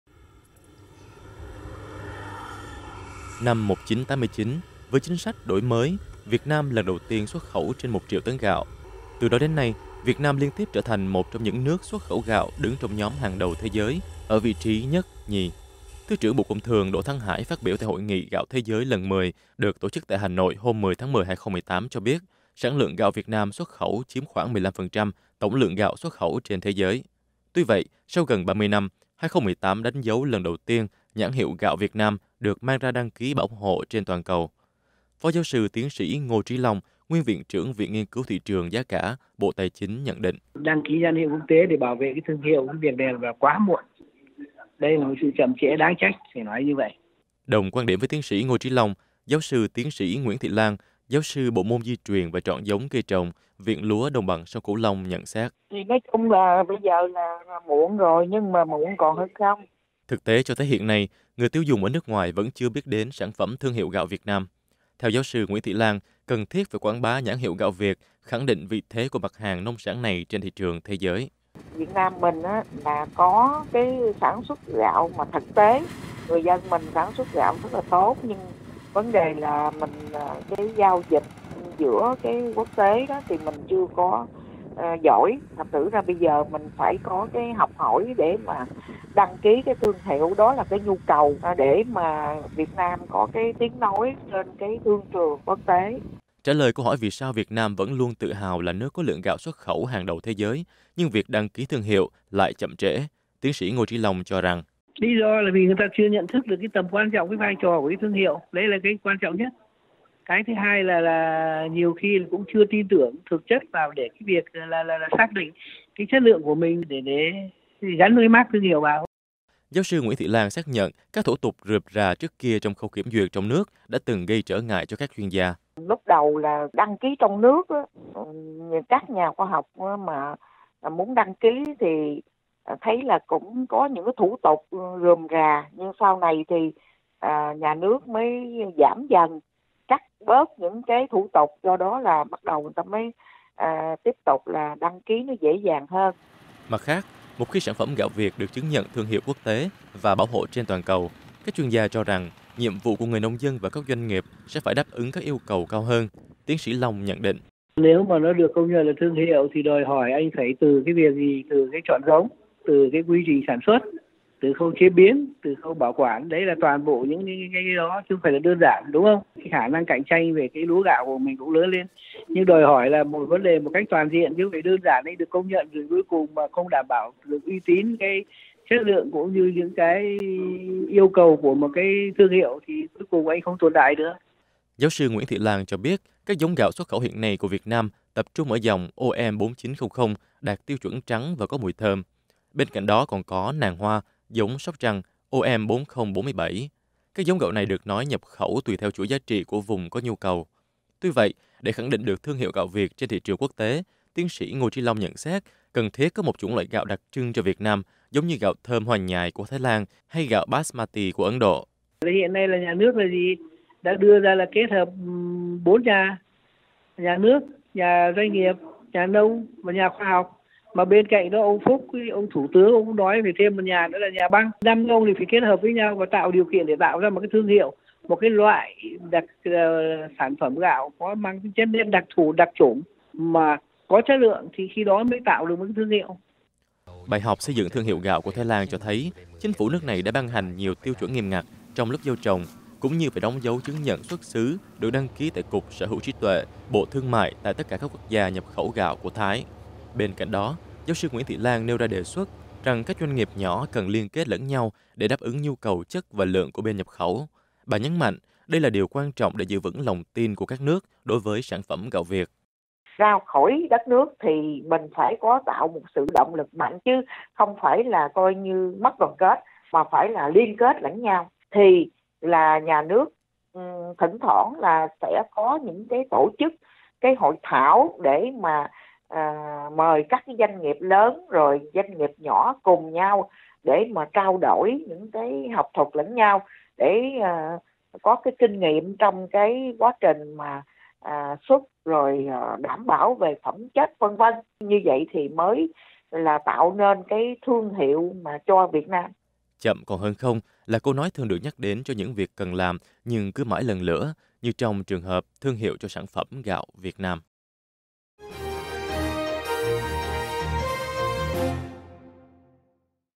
Thương hiệu gạo Việt cần sản phẩm đặc trưng
Việt Nam vừa nộp hồ sơ đăng ký nhãn hiệu quốc tế đối với gạo Việt Nam để bảo hộ trên toàn cầu. Và lễ công bố thương hiệu gạo Việt Nam sẽ diễn ra tại sự kiện Festival Lúa Gạo Lần thứ 3 được tổ chức ở Long An từ ngày 18 đến 24 tháng 12 tới đây.

Các chuyên gia nông nghiệp và kinh tế có ý kiến ra sao về việc mà truyền thông trong nước đồng loạt loan tin rằng lần đầu tiên công bố có logo thương hiệu gạo Việt Nam như thế?